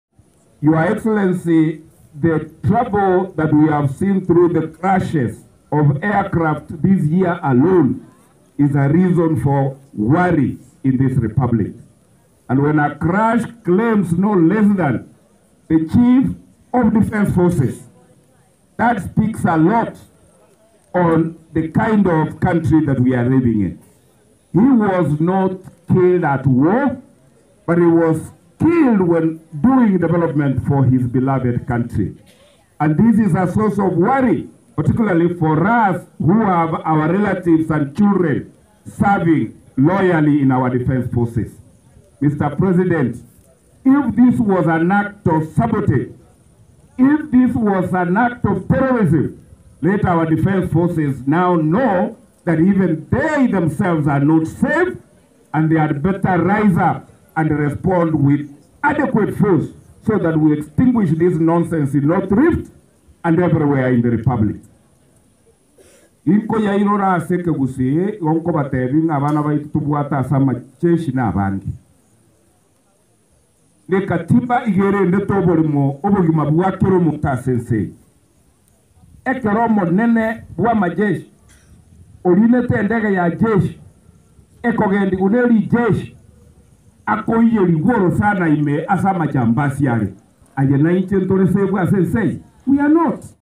Stephen Mogaka Speaks Tough On CDF Ogolla death
Stephen Mogaka |When a crash claims no less than the Chief of Defence Forces, that speaks volumes about the kind of country we are living in.